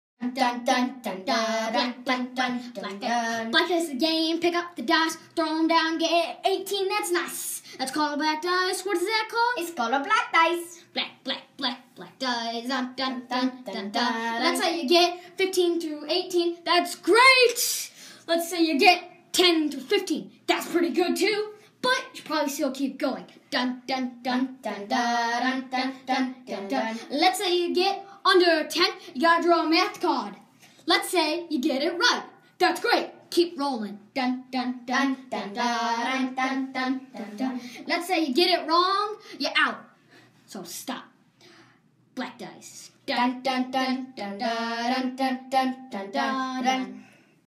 Black Dice: A Math Game
5th graders in Ms. Gittleman's Math Trailblazer's class created brand new math games-- Here's the commercial for each (and who knows, maybe they'll be at a store near you soon!)